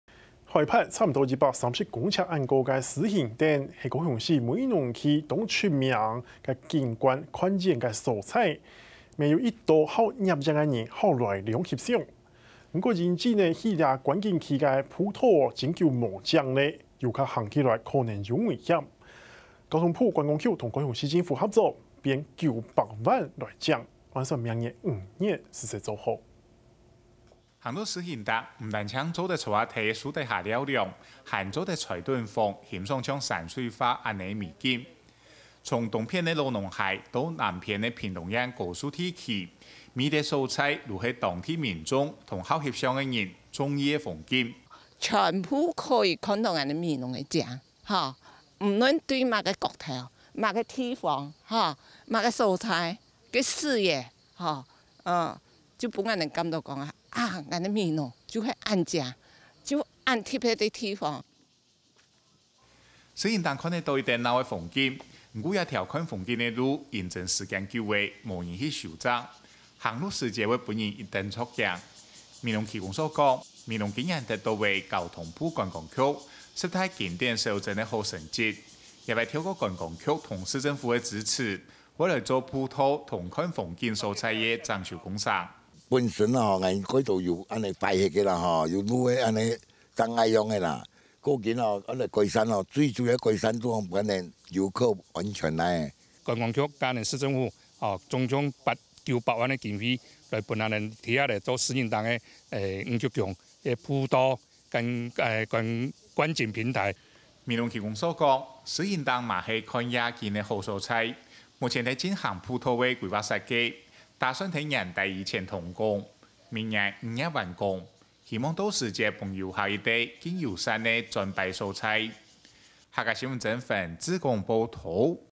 眺望美濃私房景點 「獅形頂」步道重新整修【客家新聞20190924】
海拔約130公尺的「獅形頂」，是高雄市美濃區著名的景觀眺望點，也是不少攝影玩家喜愛的私房景點，不過由於通往觀景區的步道年久失修，遊客行走有打滑的疑慮，交通部觀光局與高雄市政府合作，編列900萬元經費整修，預計明年5月完工。